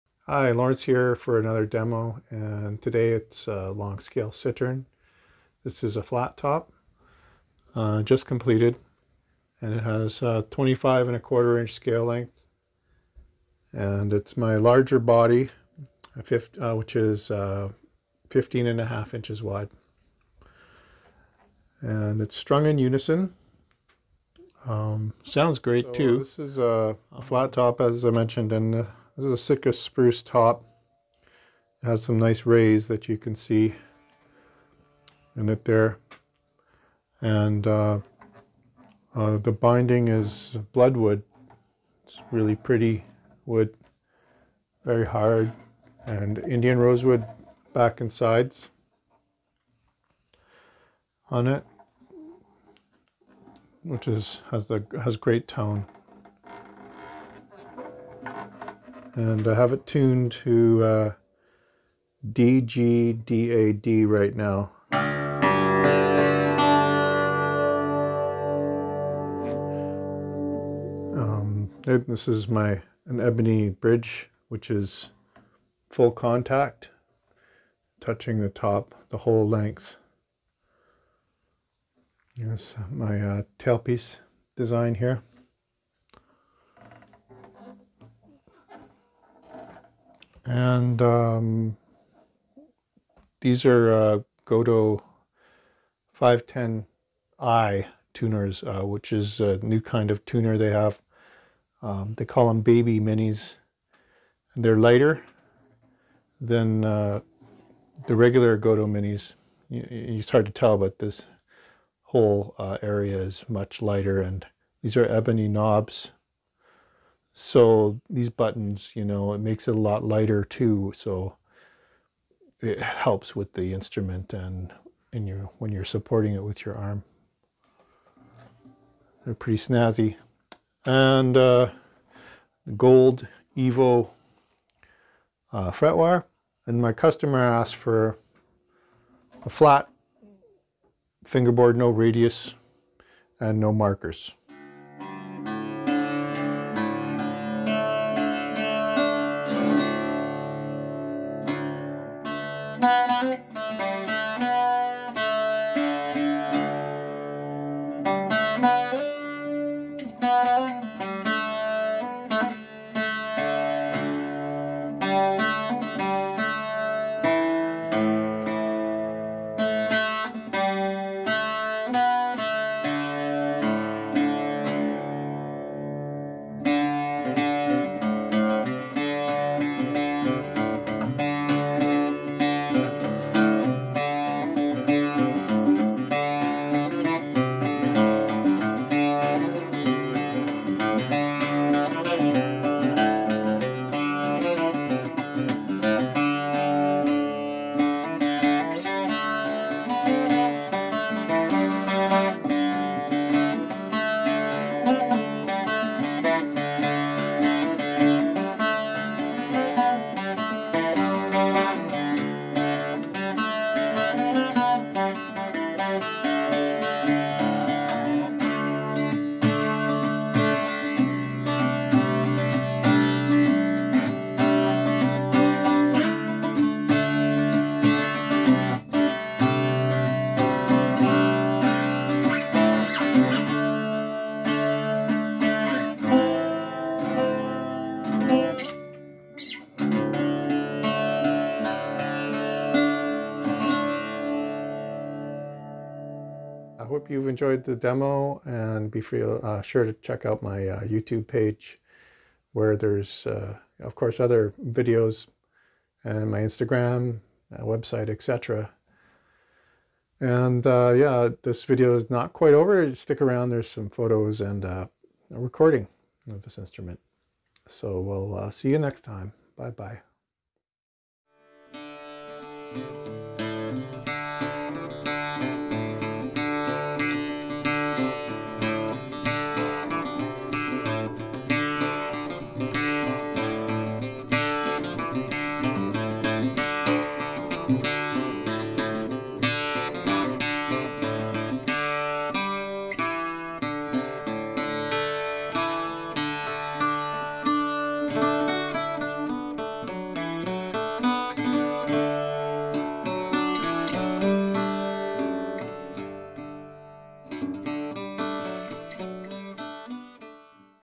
Nyberg Flat-top Long-Scale Cittern Demo ; 10 string Irish Bouzouki
25.25" scale length Cittern
Tuned in Unison DGDAD,
gauges .012 ; .018pb ; .030 ; .046 ; .056
Indian rosewood body , sitka spruce flat-top
Bloodwood binding
15.5" wide body, approx. 3 1/2" deep
gotoh gold 510i baby minis w/ebony knobs